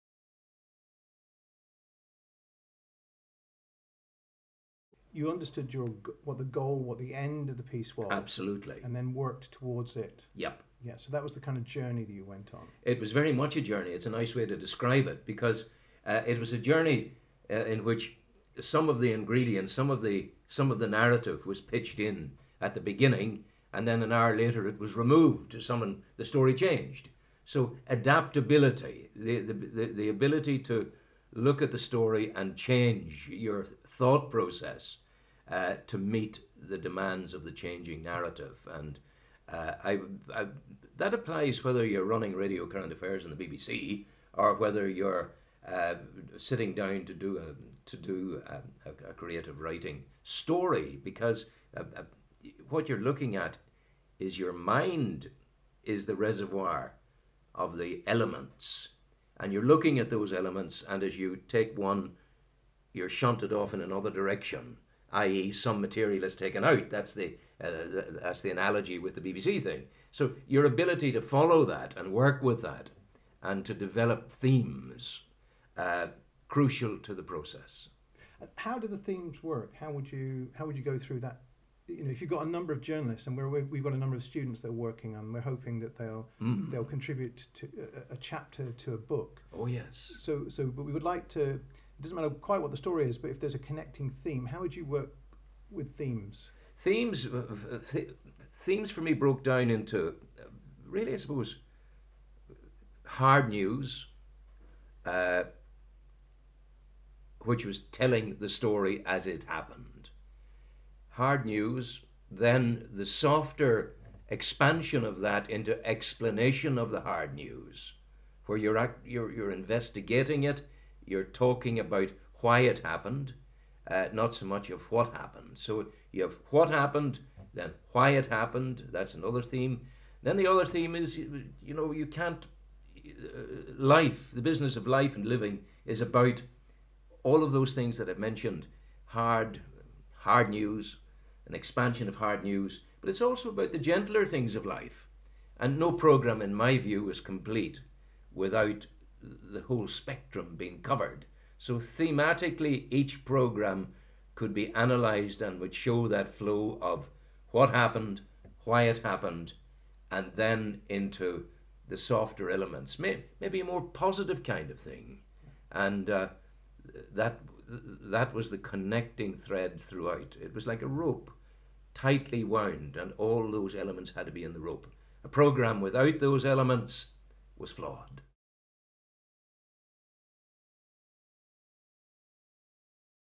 Creative Writing Skills: Ep 03 Goal Narrative
In this episode journalist and broadcaster, Rowan Hand discusses goal narrative.